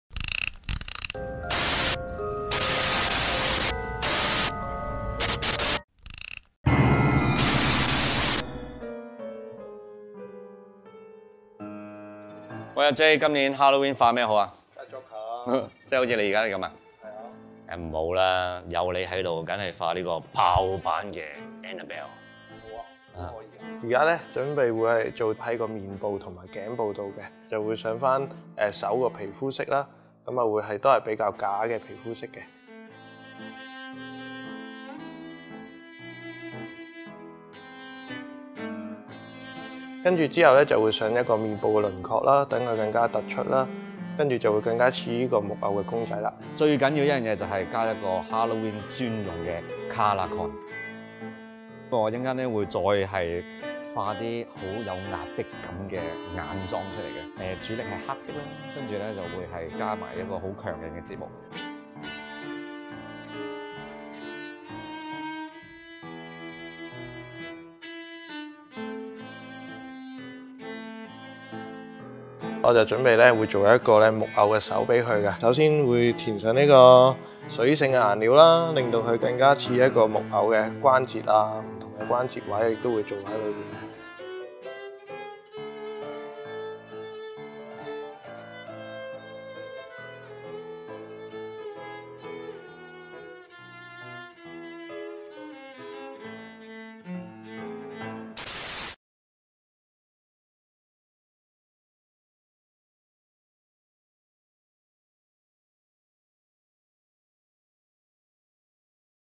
化身Annabelle！星級化妝師神還原變裝過程｜she.com
來到萬聖節，今年化身什麼造型嚇人好？這次星級化妝師 Jonathan Li 和人體彩繪師 Jay Tang 就親身示範如何化出一個「爆版Annabelle」妝容。大家又覺得這個造型夠不夠吸引呢？